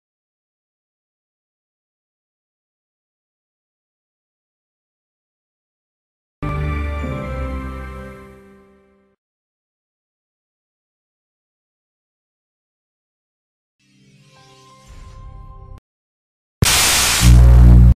Fathers and Mothers logo combo
Here is a logo combo that i made on AVS Video Editor.